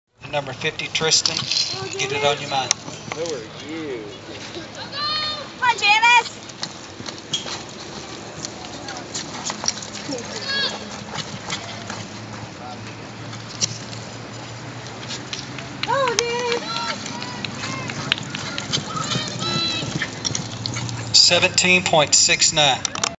Abby and I running at The Caney Creek Cowboy Church on November 30, 2013.
Barrel race in Grangerland, TX.  17.69